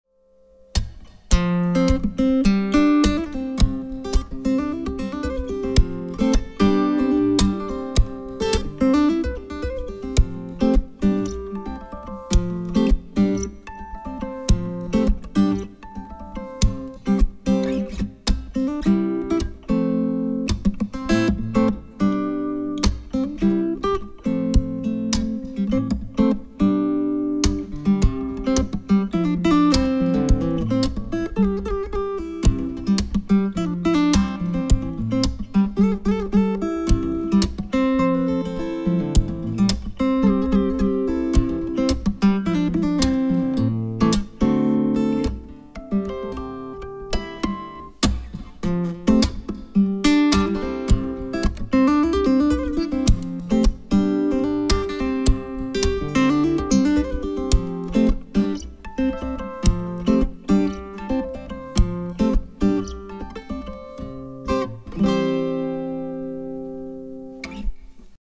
Electro Acoustic Guitar LAG T170ACE
Lag T170 is the first model on the range with a solid red cedar top meticulously chosen for its acoustic qualities. Available in dreadnought and auditorium shapes the T170 promises a deep and warm sound, which is faithfully reproduced with the Astro-Lâg preamp that appears in the acoustic-electric versions.